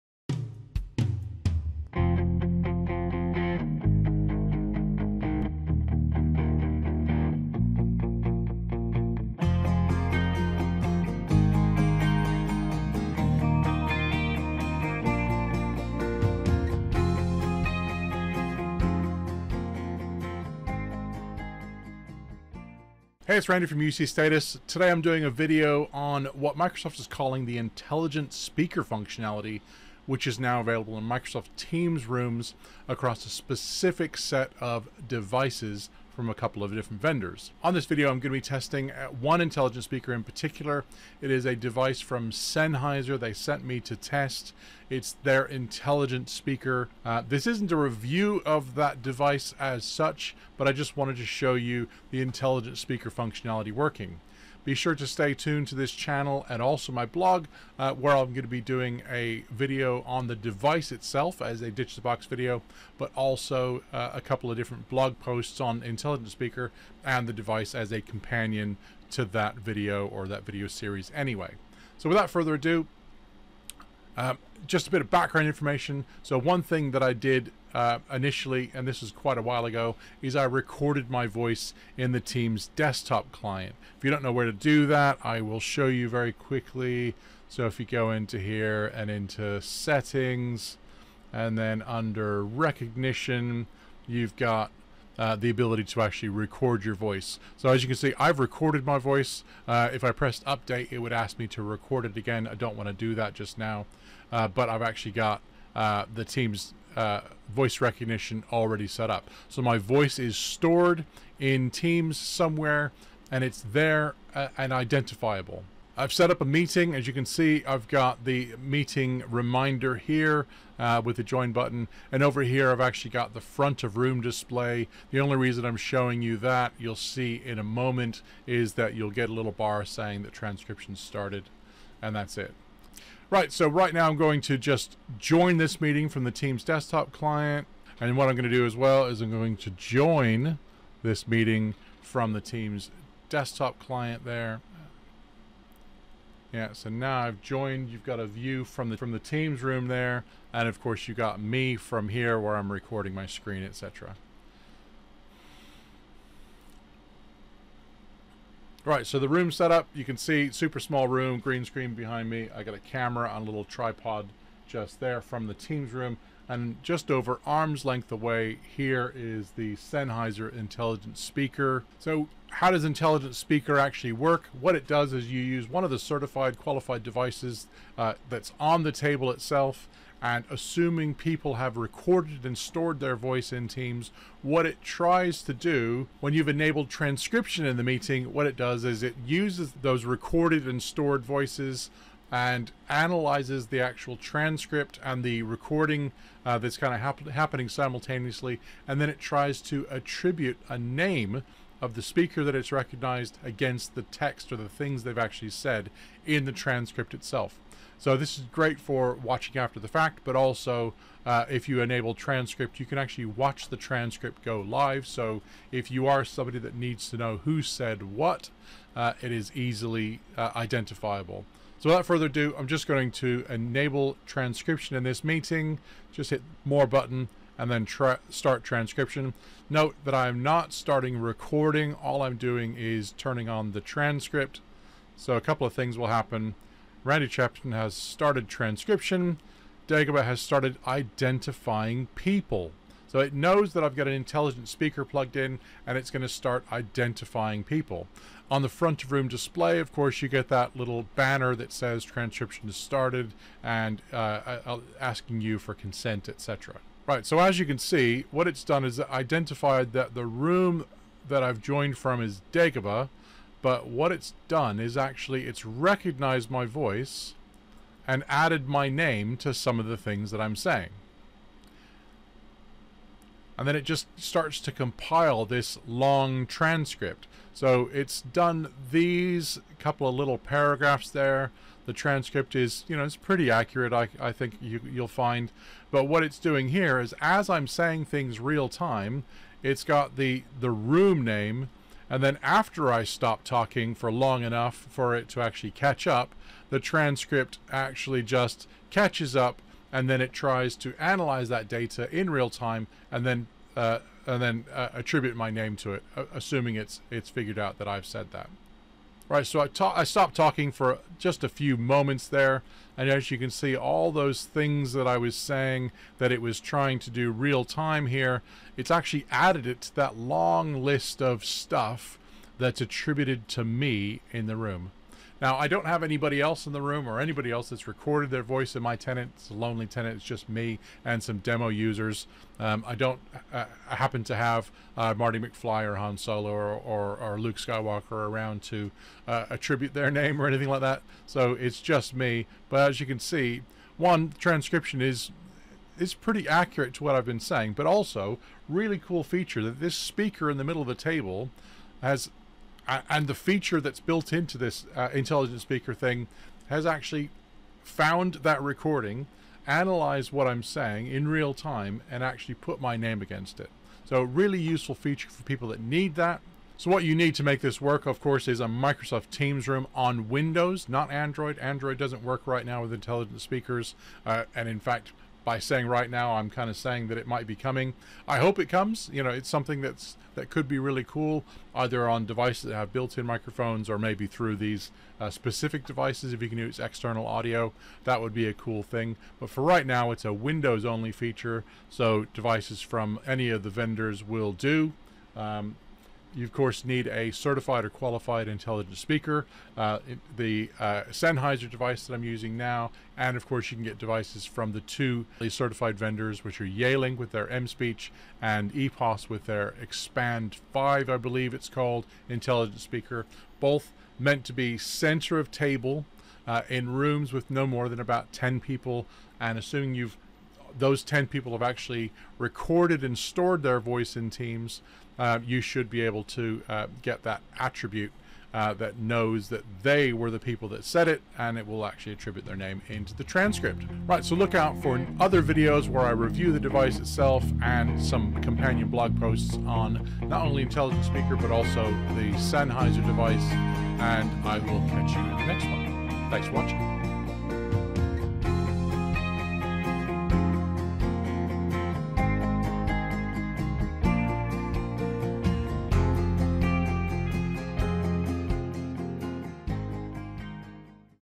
Sennheiser TeamConnect Intelligent Speaker in Microsoft Teams Rooms
In this short video I show you what Microsoft calls the Intelligent Speaker functionality in Microsoft Teams Rooms. For this I am using the Sennheiser TeamConnect Intelligent Speaker.

This isn't a review of the device, but a quick video showing you a demonstration of feature working. I will do an in depth review in another video shortly.

Here is a link to the Microsoft page with the setup